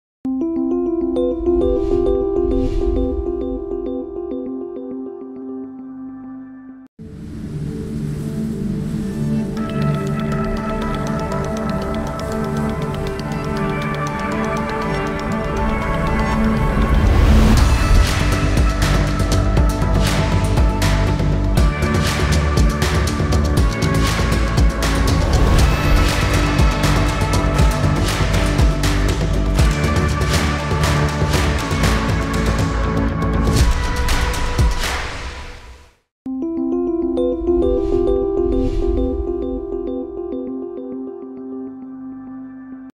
Breitling - BRAND NEW Endurance Pro IRONMAN Revealed l Jura Watches
Discover the all-new Breitling Endurance Pro IRONMAN, celebrating Breitling's partnership with IRONMAN.